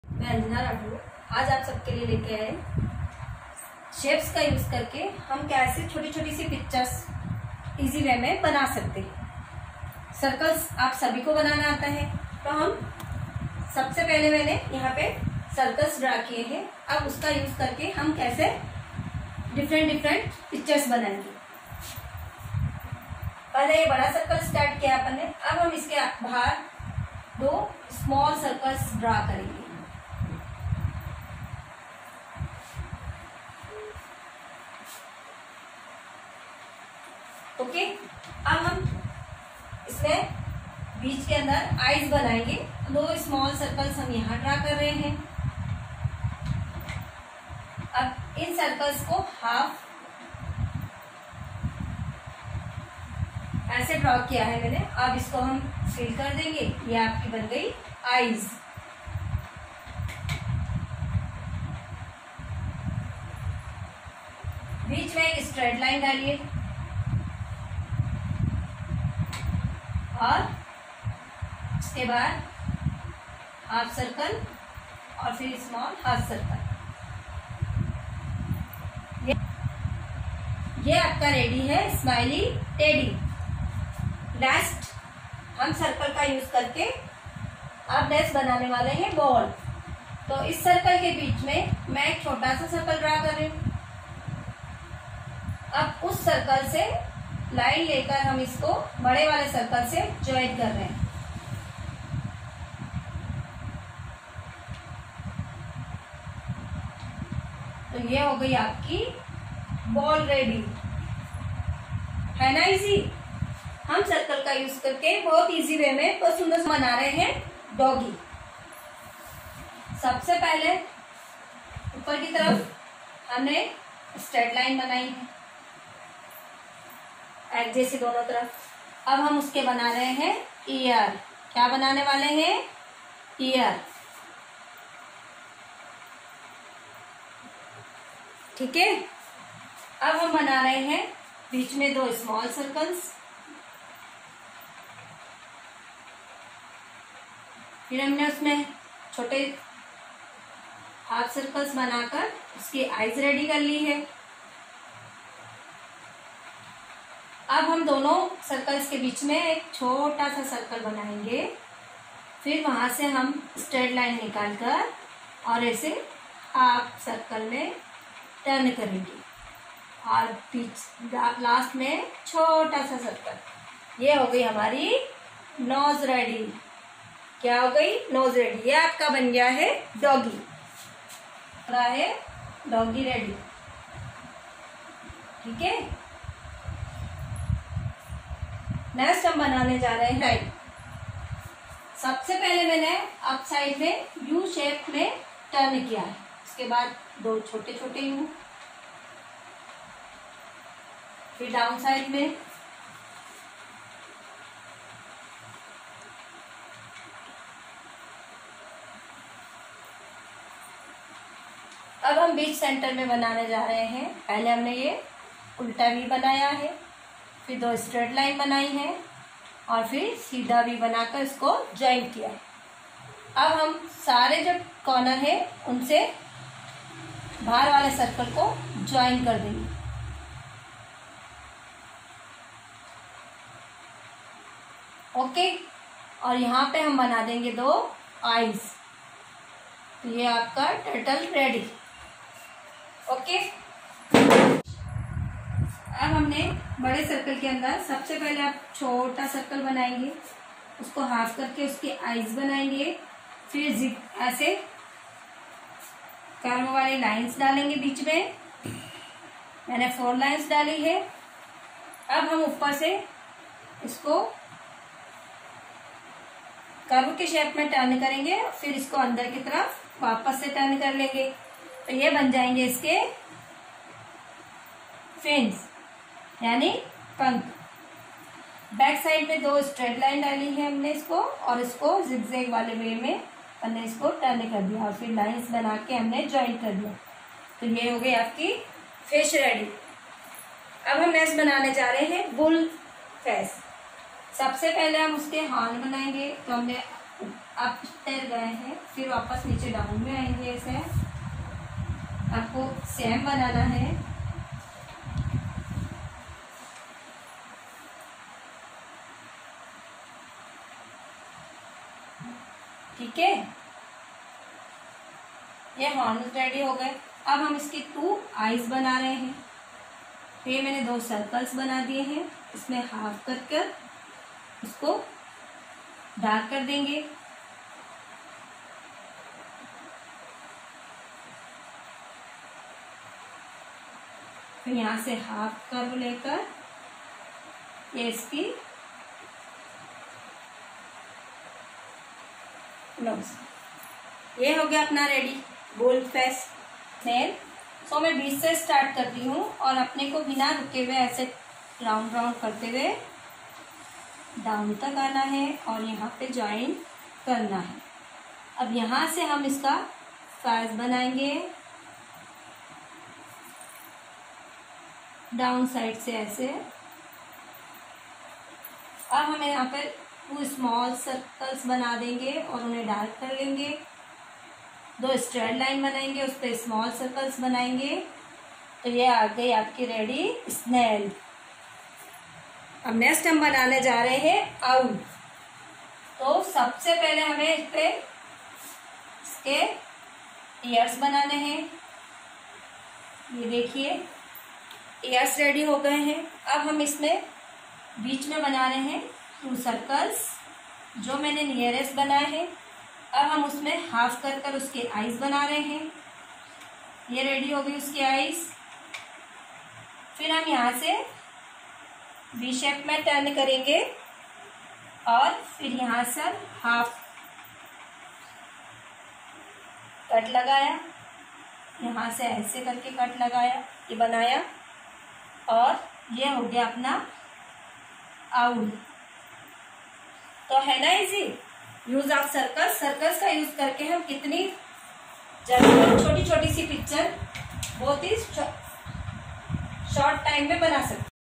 EASY TRICK TO MAKE PICTURES WITH THE HELP OF CIRCLE by Mrs Anjana Rathore from Jaora Public School
EASY TRICK TO MAKE PICTURES WITH THE HELP OF CIRCLE by Mrs Anjana Rathore from Jaora Public School, jaora Mo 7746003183